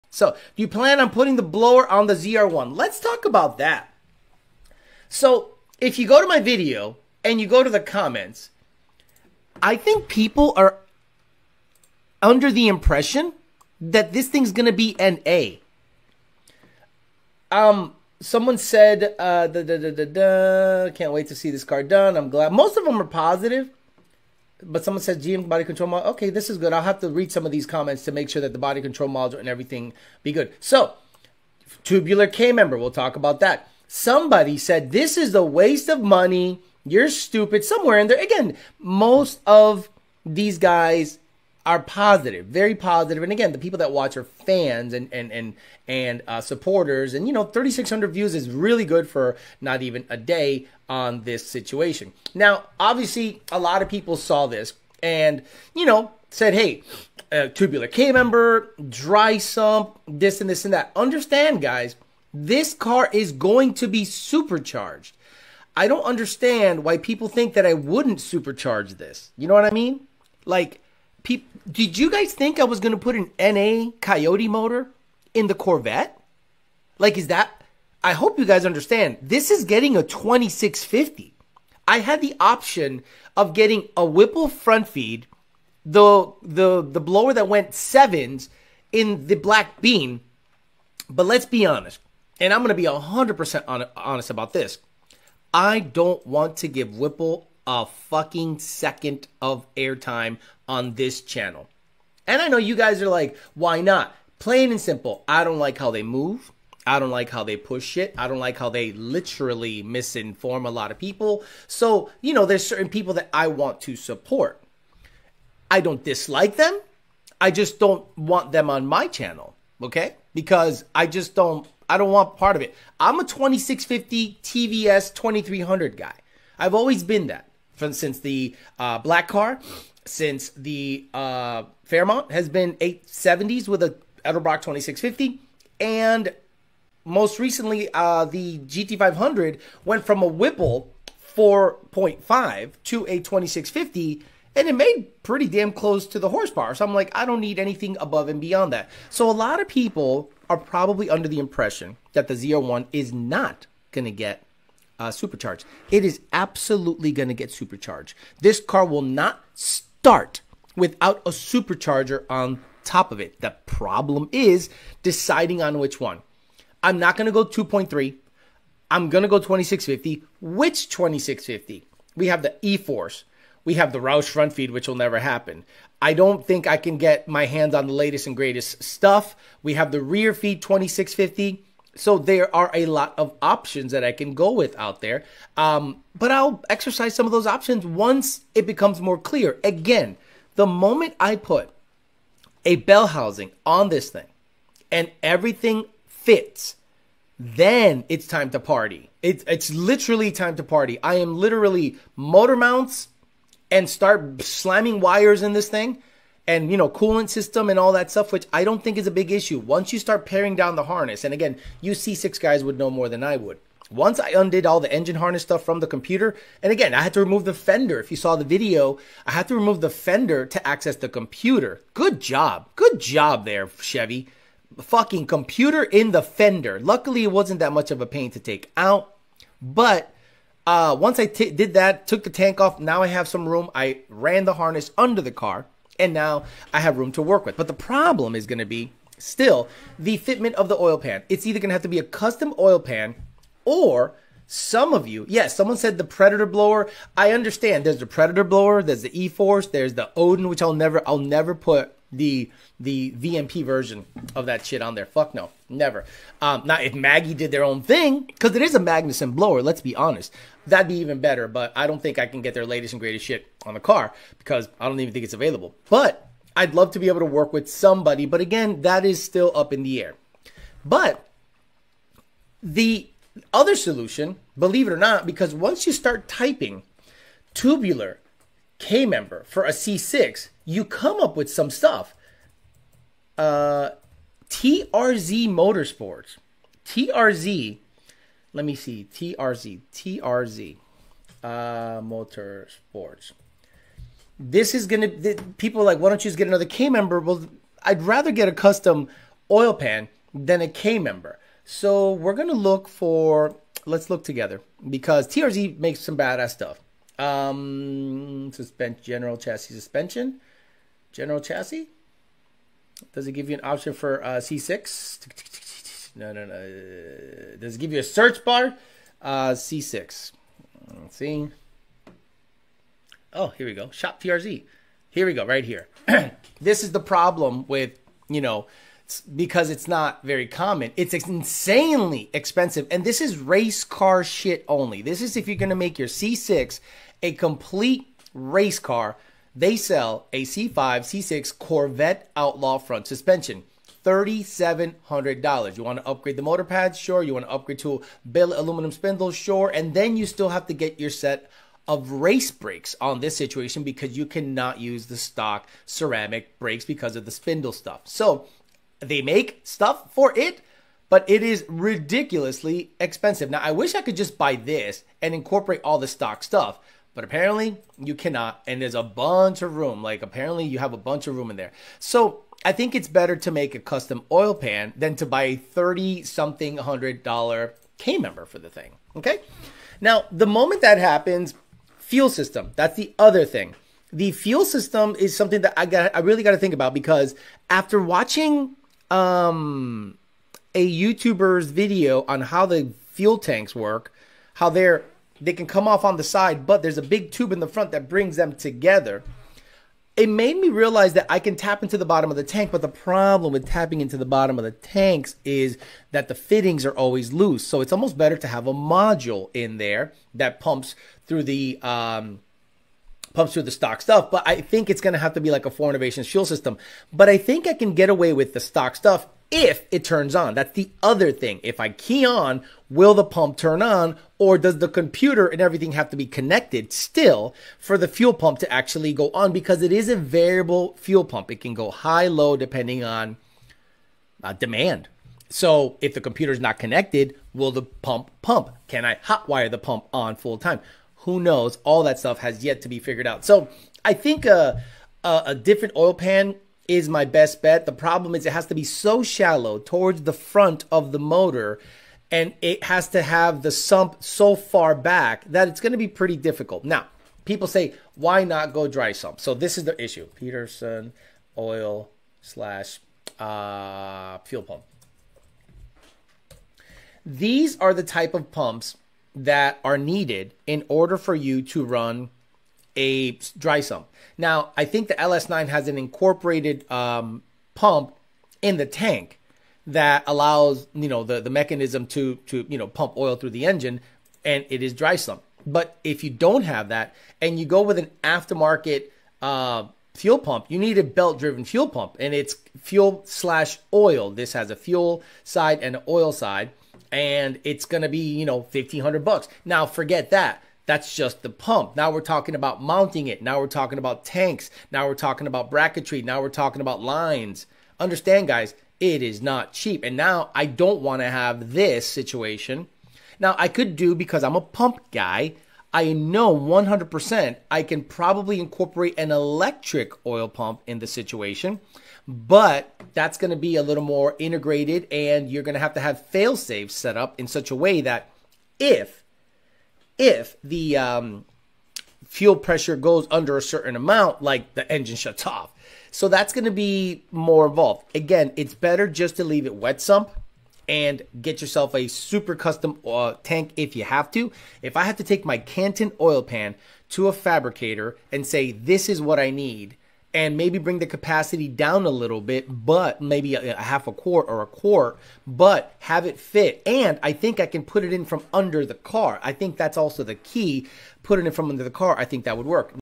The ONLY Supercharger I'll Put on My Coyote ZR1
Looking to supercharge a Coyote-swapped ZR1? We're breaking down why the 2650 TVS supercharger is the ONLY choice for this insane build. From oil pan challenges to fuel system considerations, get the inside scoop on why certain brands didn't make the cut and what it takes to make this unique combination work. Whether you're a Ford or Chevy fan, this unconventional build shows what's possible when you think outside the box. See why the 2650 TVS has proven itself time and time again in our builds, and why it's the perfect choice for this Coyote-powered Corvette. Ready to see something different? This isn't your average engine swap - it's a supercharged statement.

CHAPTERS:
00:00 - ZR1 Supercharged Discussion
02:02 - Supercharger Options for ZR1
04:33 - ZR1 Tubular K-Member Features